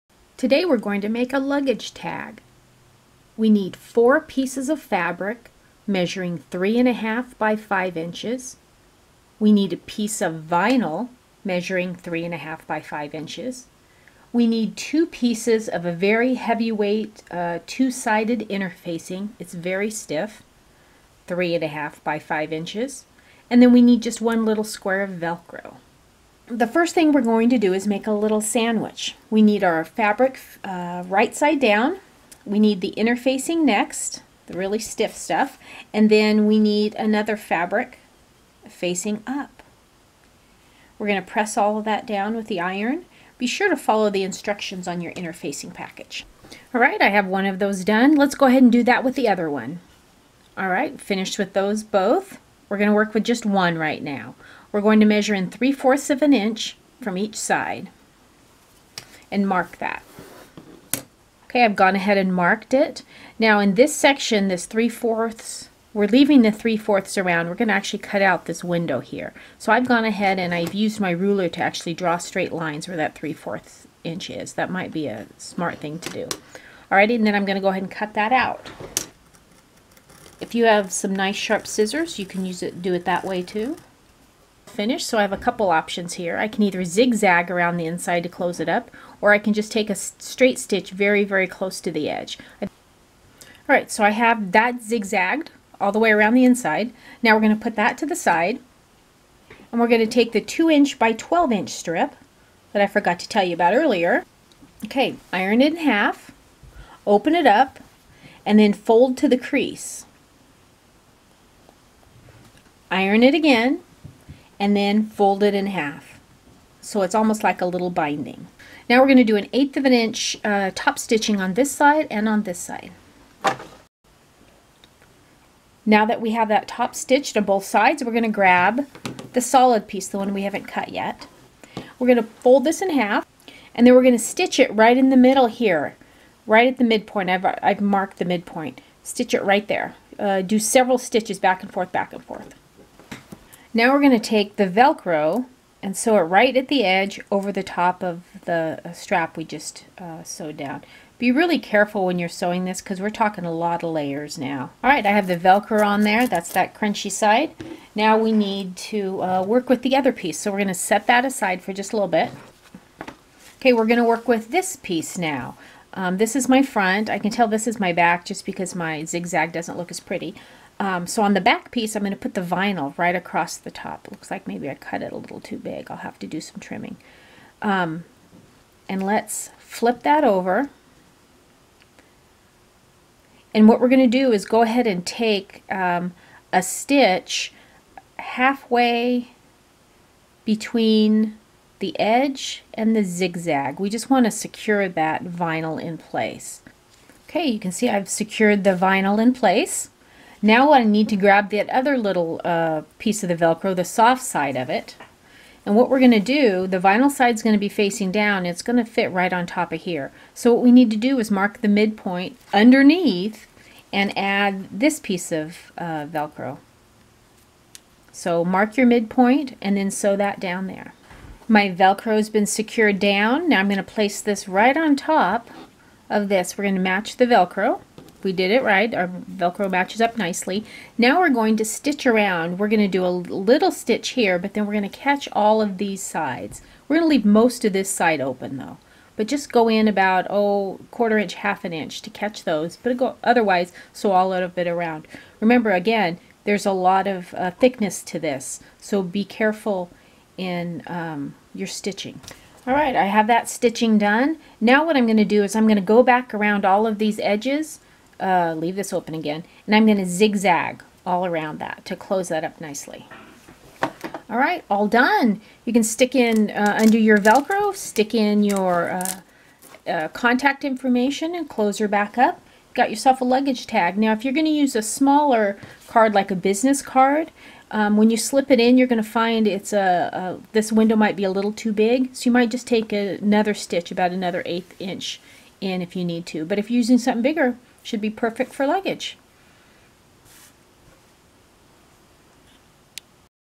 How to Sew Your Own Homemade Fabric Luggage Tag - DIY How-to Project
If you're looking for travel project ideas on how to make your own homemade fabric luggage tag with a clear vinyl window, check out this step by step tutorial. This DIY sewing project idea will help you make your own handmade baggage tag for your upcoming vacation or trip.